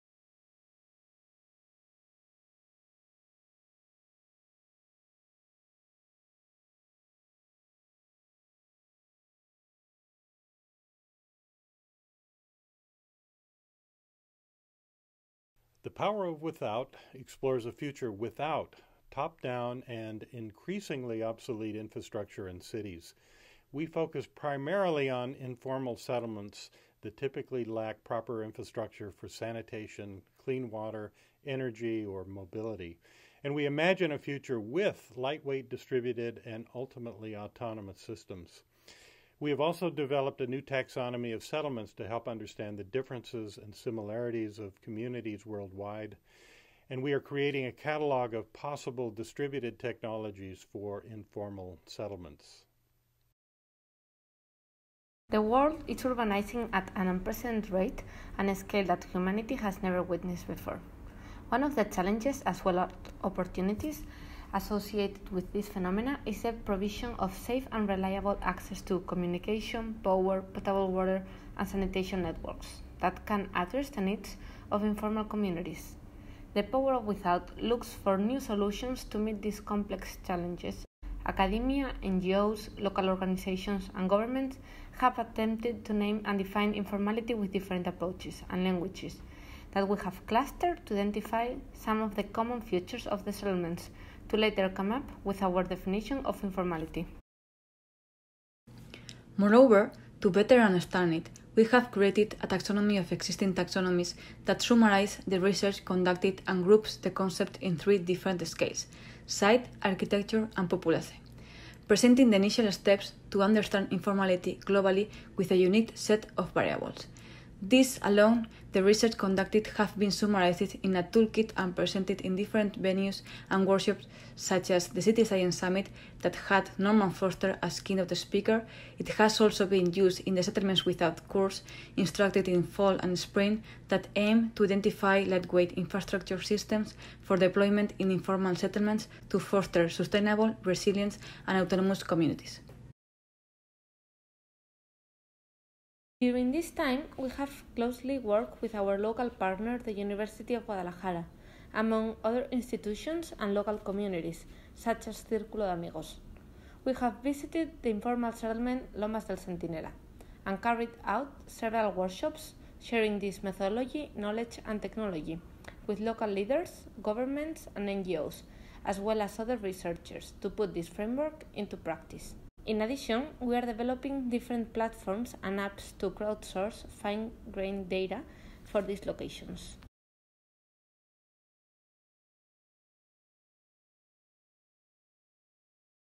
City Science Demo: The Power of Without
Team: Luis Alonso, Guadalupe Babio, Maitane Iruretagoyena, Leticia Izquierdo, Gabriela Advincula, Jason Nawyn, Margaret Church and Kent Larson

The Power of Without explores a future WITHOUT obsolete top-down infrastructure in cities. We focus primarily on informal settlements that typically lack proper infrastructure for sanitation, clean water, energy, or mobility … and imagine a future WITH lightweight, distributed and ultimately autonomous systems.